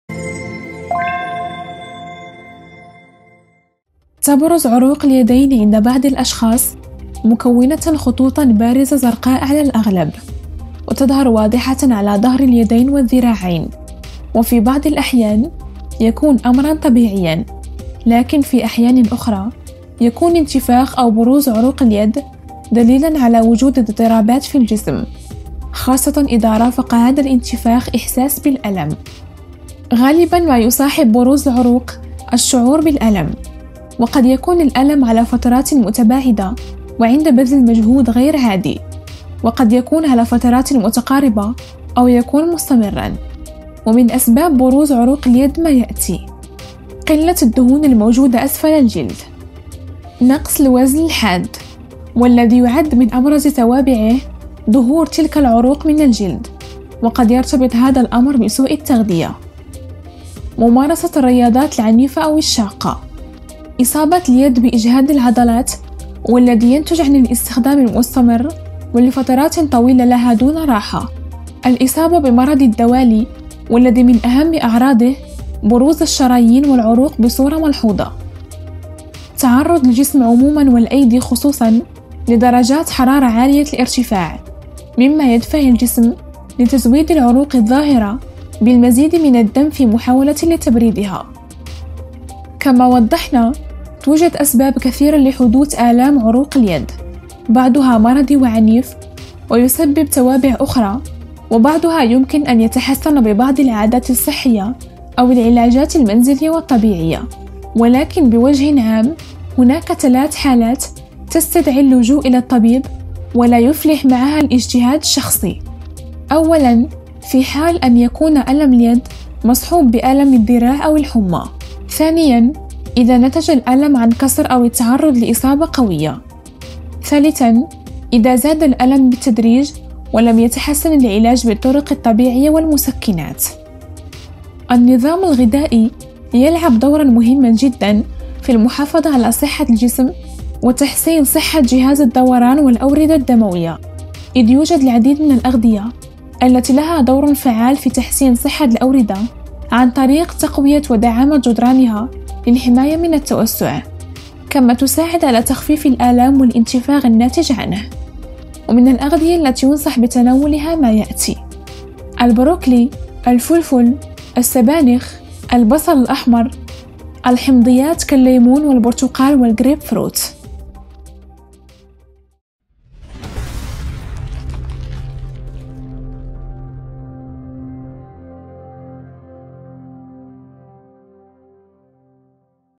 بروز عروق اليد فجأه مع الم
تبرز عروق اليدين عند بعض الأشخاص مكونة خطوطا بارزة زرقاء على الأغلب، وتظهر واضحة على ظهر اليدين والذراعين  وفي بعض الأحيان يكون أمرًا طبيعيًّا، لكن في أحيان أخرى يكون انتفاخ أو بروز عروق اليد دليلًا على وجود اضطرابات في الجسم، خاصةً إذا رافق هذا الانتفاخ إحساس بالألم،